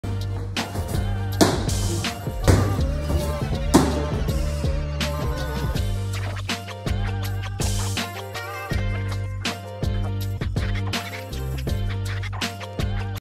Change Up Velocity 4K
Watch as Pitching Ninja, Kyle Boddy, and Cutternation bring you some of the best live at-bats from Dallas, Texas! From viral curveballs to TikTok sports, this video has it all and guarantees to rack up views. Get ready to witness some of the biggest hits and impressive pitches in Texas!

CNBSB
CutterNation
AXIO Trainer